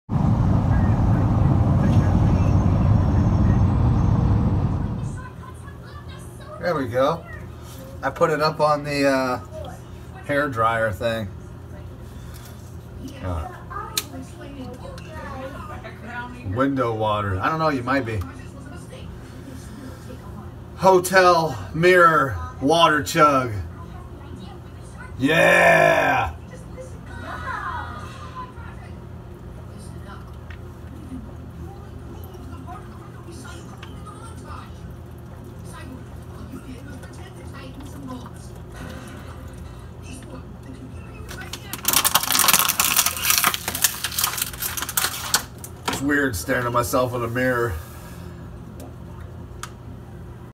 Traveling Across America Hotel Room Water Chug Video 55
Hotel Room

Across America

ASMR Gulping Sounds Chugging a water bottle before bed every night for a year Video 55 16.9 ozs of H2O